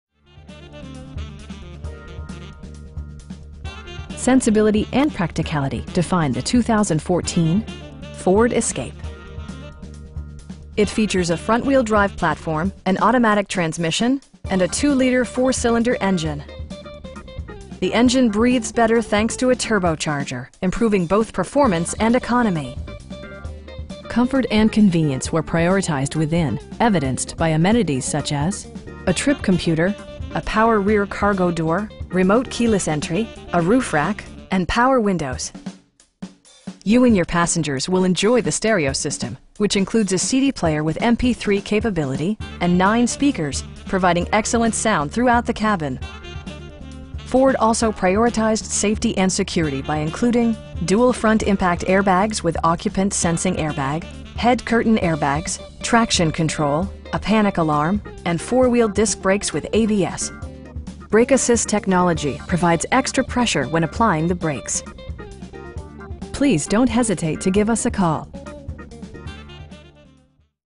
2014 Ford Escape SE in Santa Rosa, CA 95407
Hansel Ford

Sensibility and practicality define the 2014 Ford Escape. It features a front-wheel-drive platform, an automatic transmission, and a 2 liter 4 cylinder engine. The engine breathes better thanks to a turbocharger, improving both performance and economy. Comfort and convenience were prioritized within, evidenced by amenities such as: a trip computer, tilt and telescoping steering wheel, a power rear cargo door, remote keyless entry, cruise control, a roof rack, and power windows. You and your passengers will enjoy the stereo system, which includes a CD player with MP3 capability, and 9 speakers, providing excellent sound throughout the cabin. Ford also prioritized safety and security by including: dual front impact airbags with occupant sensing airbag, head curtain airbags, traction control, a panic alarm, and 4 wheel disc brakes with ABS. Brake assist technology provides extra pressure when applying the brakes. Our aim is to provide our customers with the best prices and service at all times. Please don't hesitate to give us a call.